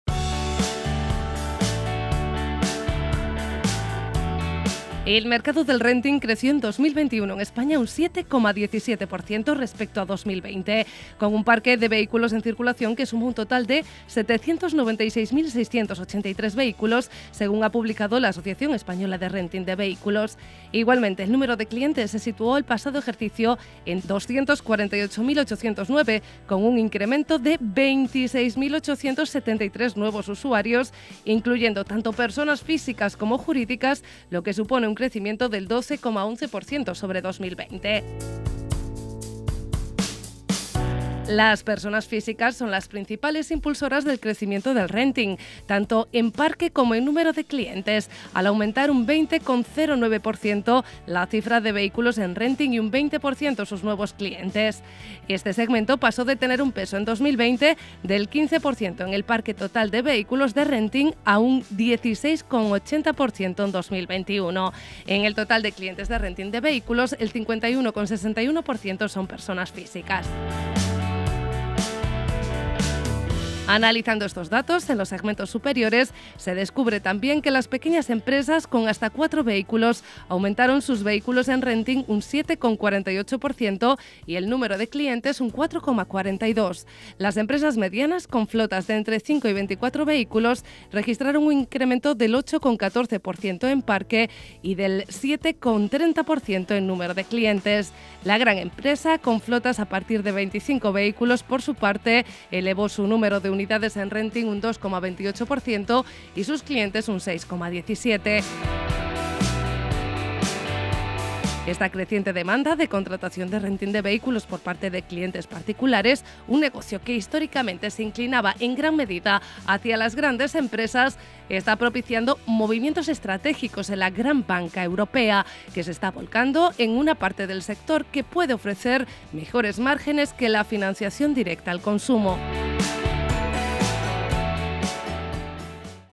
EL ‘RENTING’ CRECE EN ESPAÑA MÁS DE UN 7% EN 2021 | By CdRas
El mercado del renting creció en 2021 en España un 7,17%, respecto a 2020, con un parque de vehículos en circulación que suma un total de 796.683 vehículos, según ha publicado la Asociación Española de Renting de Vehículos. Igualmente, el número de clientes se situó el pasado ejercicio en 248.809, con un incremento de 26.873 nuevos usuarios, incluyendo tanto personas físicas como jurídicas, lo que supone un crecimiento del 12,11% sobre 2020.
Las personas físicas son las principales impulsoras del crecimiento del renting, tanto en parque como en número de clientes, al aumentar un 20,09% la cifra de vehículos en renting y un 20% sus nuevos clientes. Este segmento pasó de tener un peso, en 2020, del 15% en el parque total de vehículos de renting, a uno del 16,80%, en 2021. En el total de clientes de renting de vehículos, el 51,61% son personas físicas.
Analizando estos datos en los segmentos superiores se descubre, también, que las pequeñas empresas, con hasta cuatro vehículos, aumentaron sus vehículos en renting un 7,48% y el número de clientes un 4,42%. Las empresas medianas, con flotas de entre 5 y 24 vehículos, registraron un incremento del 8,14%, en parque y del 7,30%, en número de clientes. La gran empresa, con flotas a partir de 25 vehículos, por su parte, elevó su número de unidades en renting un 2,28% y sus clientes, un 6,17%.
Esta creciente demanda de contratación de renting de vehículos por parte de clientes particulares, un negocio que históricamente se inclinaba en gran medida hacia las grandes empresas, está propiciando movimientos estratégicos en la gran banca europea, que se está volcando en una parte del sector que puede ofrecer mejores márgenes que la financiación directa al consumo.
La principal noticia se ha producido a remolque de la creación de Stellantis, que aspira a duplicar su flota de coches en renting durante la próxima década. Para ello, ha llegado a un acuerdo con la banca francesa Crédit Agricole, por el que Crédit Agricole Consumer Finance y Stellantis prevén la creación de una unidad de leasing independiente, mediante la adquisición por parte de Crédit Agricole Consumer Finance del 100% del capital de FCA Bank y Leasys Rent.
El acuerdo prevé la creación, por un lado, de un líder paneuropeo en el arrendamiento de vehículos a largo plazo mediante la agrupación de Leasys y Free2Move Lease. Actualmente, Leasys es la filial de leasing de FCA Bank y Free2Move Lease, que opera la actividad de leasing de Stellantis para las marcas del anterior grupo automovilístico PSA, en el que ya se había integrado Opel.
La nueva empresa conjunta de renting multimarca prestará servicio a todos los clientes, tanto empresas como particulares, en diez países europeos, y tiene como objetivo desarrollar una flota de más de un millón de vehículos para finales de 2026 y 1,4 millones para 2030, lo que les permitirá convertirse inmediatamente en uno de los cinco principales actores en Europa. Stellantis, que cuenta con casi 700.000 vehículos en su flota de renting, va por detrás de Volkswagen, así como de ALD, LeasePlan y Arval, que está respaldada por BNP Paribas.